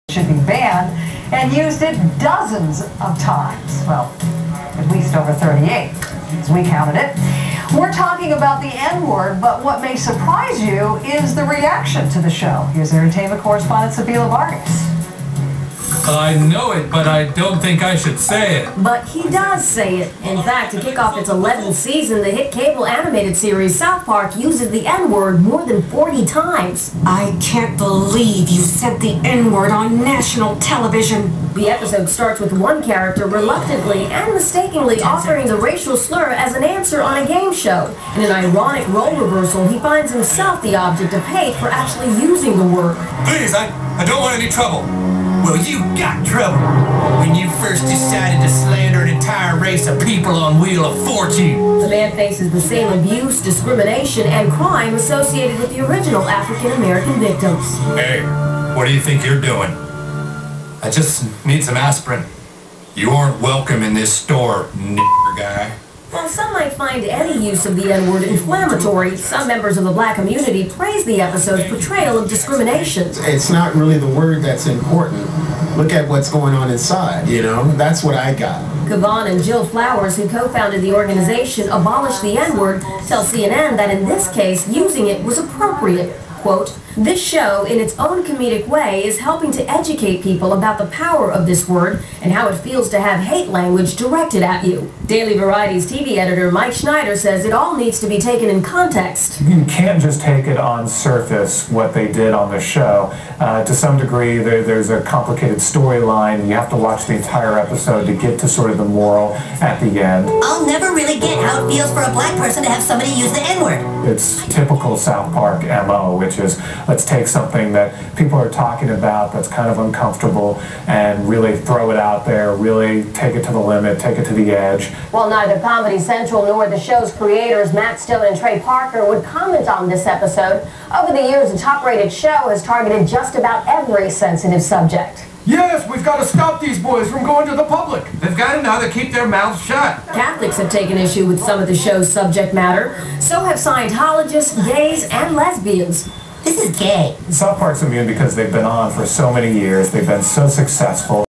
CNN Comments on the SouthPark "N" Word Episode
CNN Comments about this episode, sorry for the crappy quality :(

BTW YouTube deleting the actual clip was bullshit